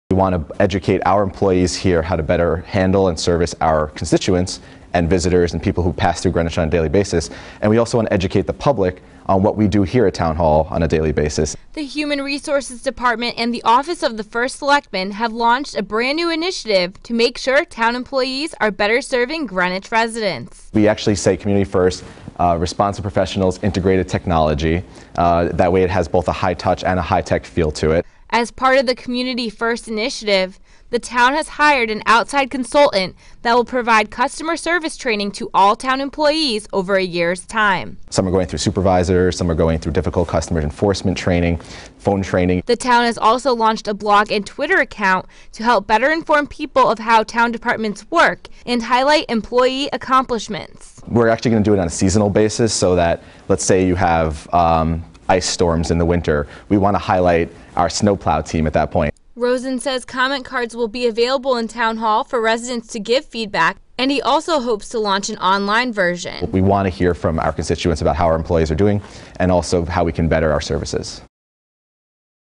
Putting the "Community First"
Community First is a new initiative put together by the town of Greenwich Human Resources Department and the Office of the First Selectman. The goal of the initiative to improve customer service throughout the town's departments and to keep the public informed about town departments through a new blog and twitter account.

Produced By: Ali Warshavsky